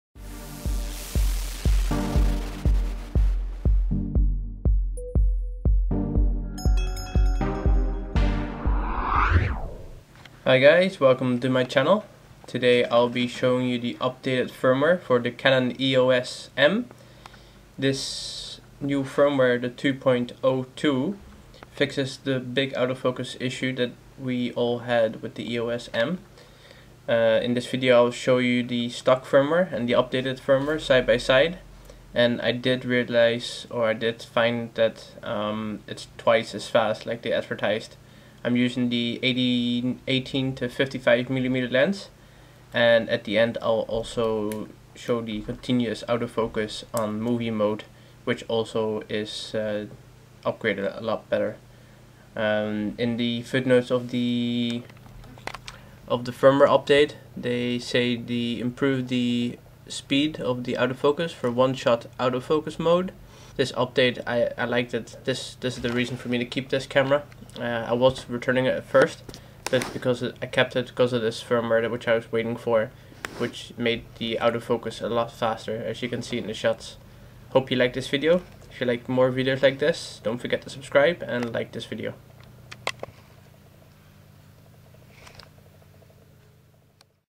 Canon EOS M - Autofocus FIX Firmware Update
Canon fixed the auto focus issue in their Canon EOS M mirrorless camera, the firmware 2.0.2 increased the autofocus speed by more than 2x. In this video I will show you side by side before and after the firmware update.